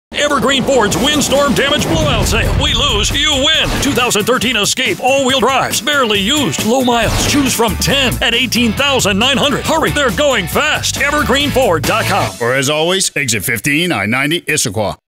ESCAPE Video
If a few scratches or dings won't steer you away, come down and save big on these 2013 Escapes!